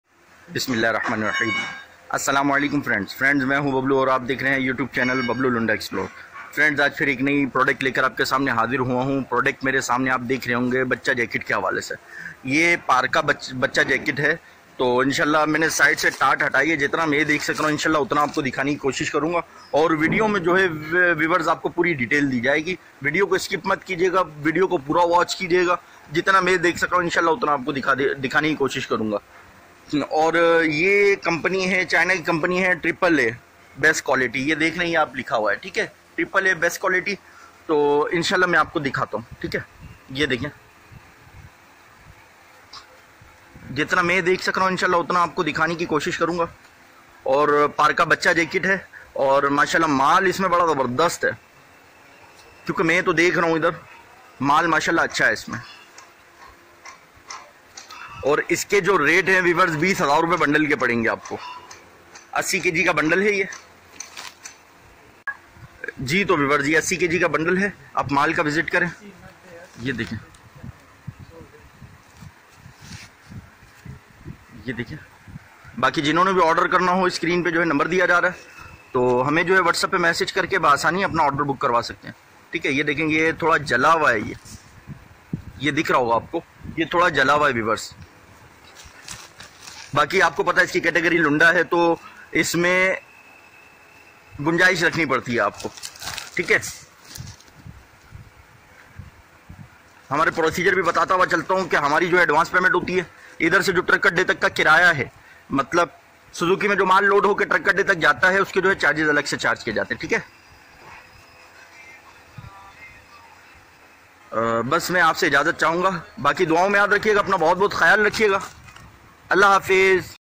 China AAA Bacha Parka Jackets 80 kg Bundle | Rs.20000 | Kids Parka Jackets | SherShah Lunda Market
China AAA Bacha Parka Jackets 80 kg Bundle | Rs.20000 | Kids Parka Jackets | SherShah Lunda Market on Wholesale